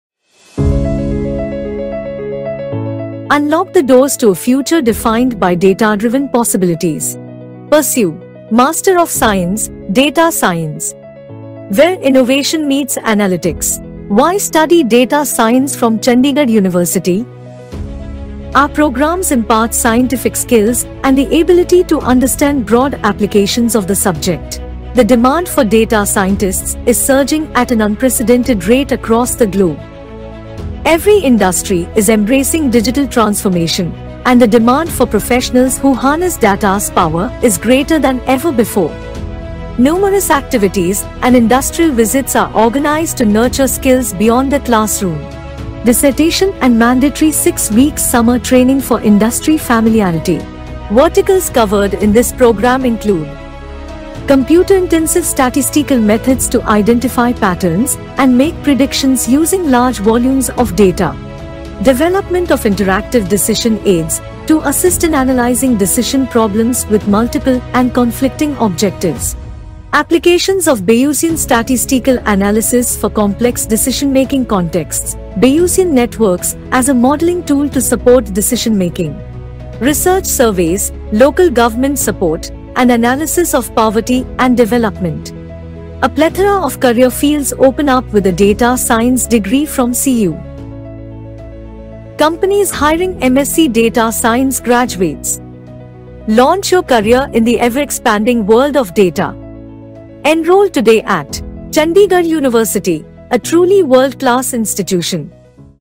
Chandigarh University M.Sc. Data Science Admission | Placements | Scholarships
Passionate about ​u​unraveling hidden patterns in data and shaping future decisions with evidence-based insights? Chandigarh University's M.Sc. in Data Science is your bespoke path to excel in the dynamic realm of data analytics.

Join one of the nation's premier Data Science programs at NAAC A+ Chandigarh University, proudly India's #1 QS World Ranked Private University.

Don't miss out! Apply through CUCHD to unlock up to 100% merit-based scholarship​ worth Rs 170 Crores.